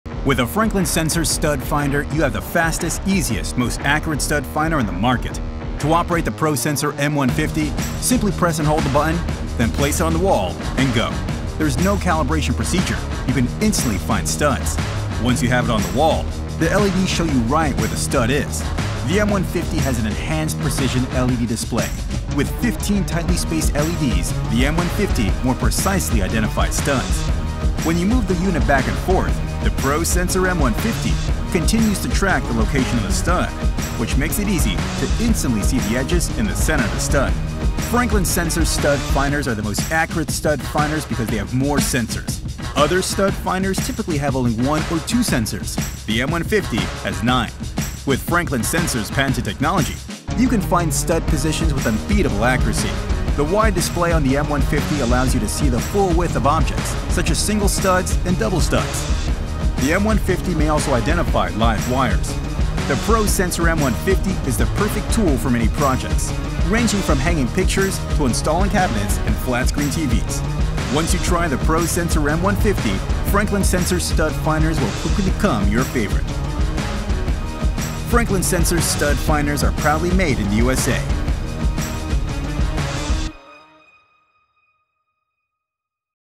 Franklin Sensors ProSensor M150 - Official Video
Franklin Sensors stud finders are the fastest, easiest, most accurate stud finders on the market.

The ProSensor M150 has 9 precision sensors while conventional stud finder have just 1 or 2 sensors. With more sensors the ProSensor M150 more accurately identifies the location of studs.

To operate, simply press and hold the button to instantly detect studs. No calibration required.

The Live Electrical Warning works continually while the button is depressed. When a live electrical wire is detected, the yellow lightning bolt symbols will illuminate to indicate the presence of live wires.